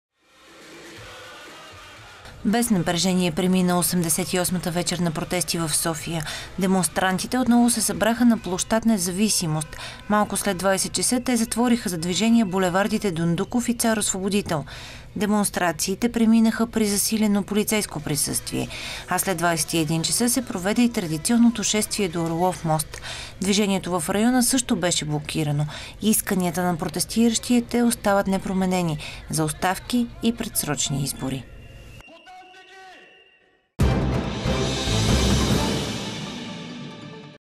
Без напрежение премина 88-ата вечер на протести в София
Демонстрантите отново се събраха на площад „Независимост”. Малко след 20 часа те затвориха за движение булевардите „Дондуков” и „Цар Освободител”.